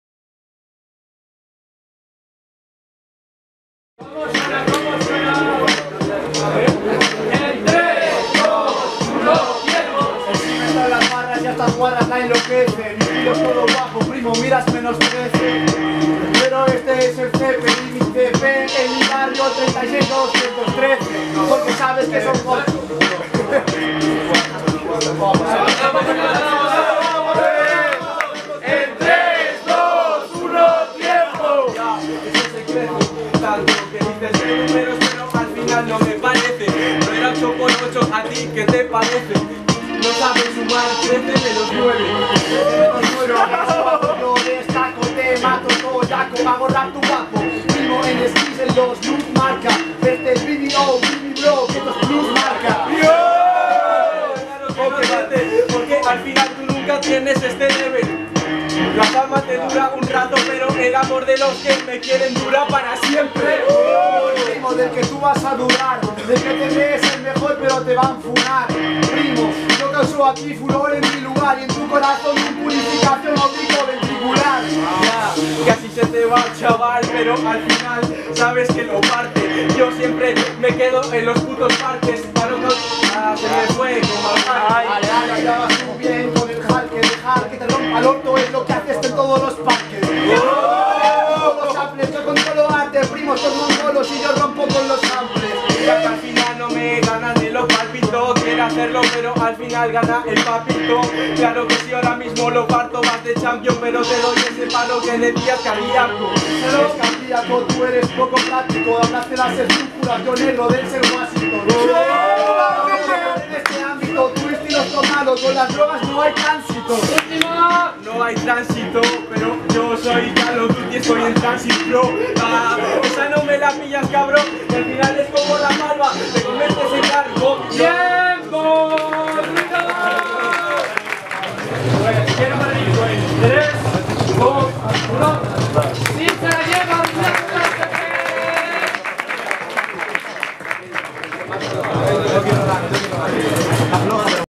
(16avos) SIMM VS CP | PRE GOLD FERROL
Uno de los vídeos de la Pre gold battle ferrolana